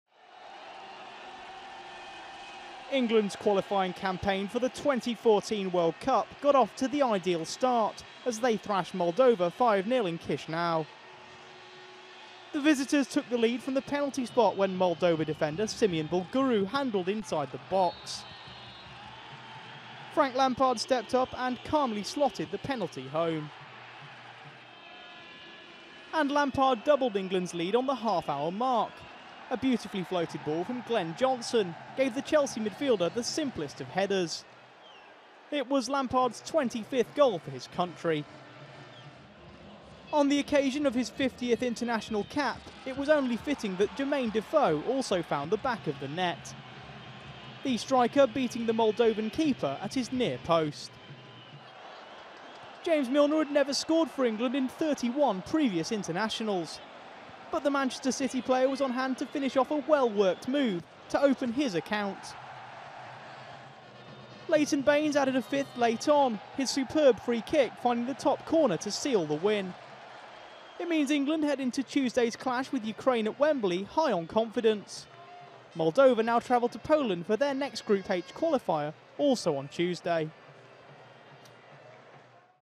England score FIVE to thrash Moldova in World Cup qualifier
Rampant England kicked off their World Cup 2014 qualifying in style with a 5-0 win over Moldova.

Roy Hodgson's men were never in trouble in Chisinau as Frank Lampard's third-minute penalty set the Three Lions on their way.

Lampard scored his second on 29 minutes and Jermain Defoe added a third two minutes later as England ran riot.

James Milner and Leighton Baines added to the scoreline after the break, as England gave Hodgson a dream qualifying start.